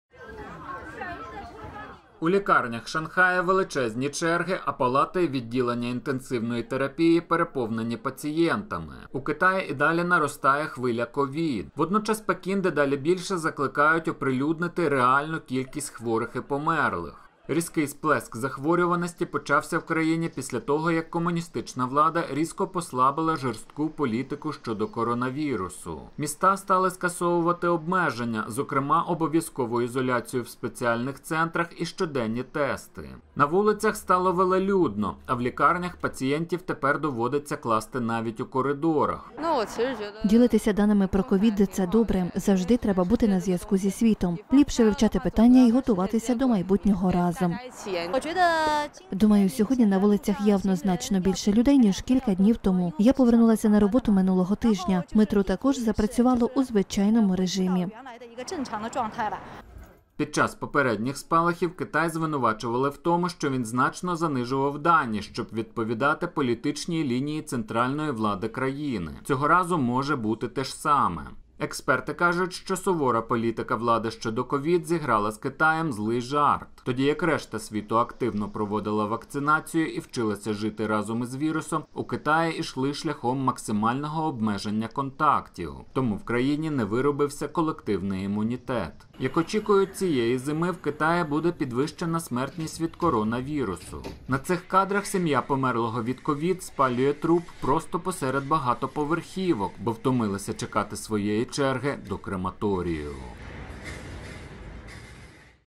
Шанхайські лікарні переповнені хворими на COVID
У лікарнях Шанхая — величезні черги, а палати й відділення інтенсивної терапії переповнені пацієнтами. У Китаї і далі наростає хвиля COVID. Водночас Пекін дедалі більше закликають оприлюднити реальну кількість хворих і померлих.